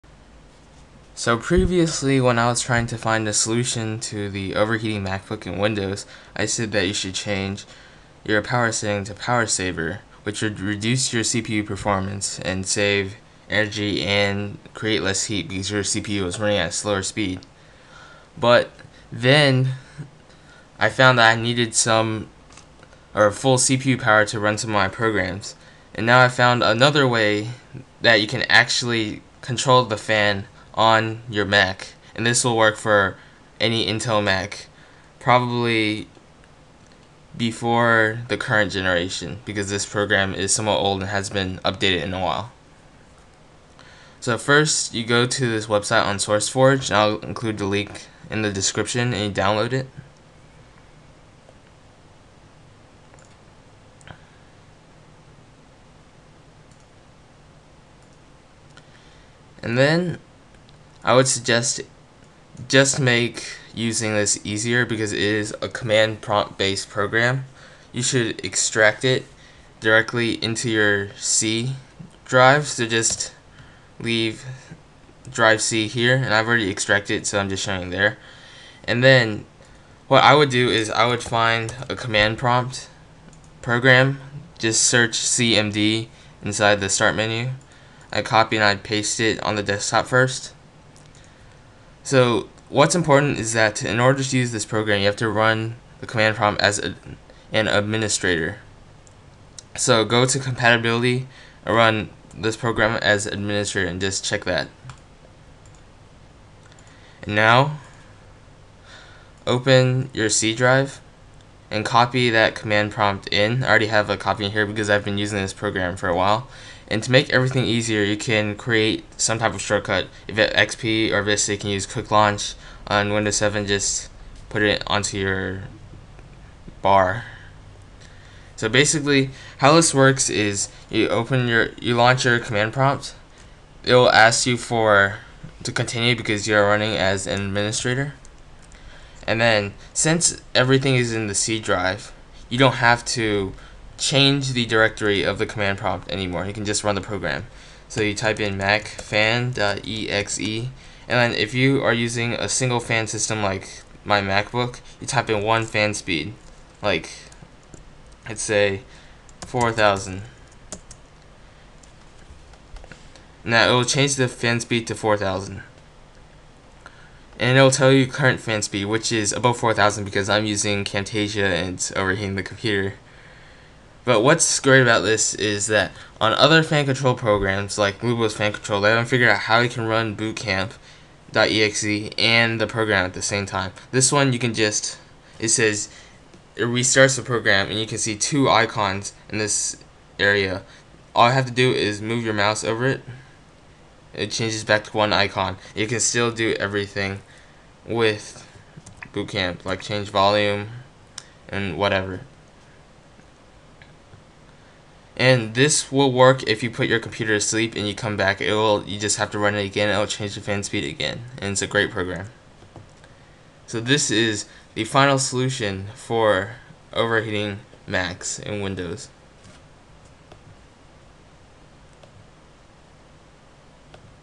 Solution: Fan Control for Overheating Macs in Windows/Boot Camp
This is a great program that allows for full fan control of your Mac and still lets bootcamp.exe work.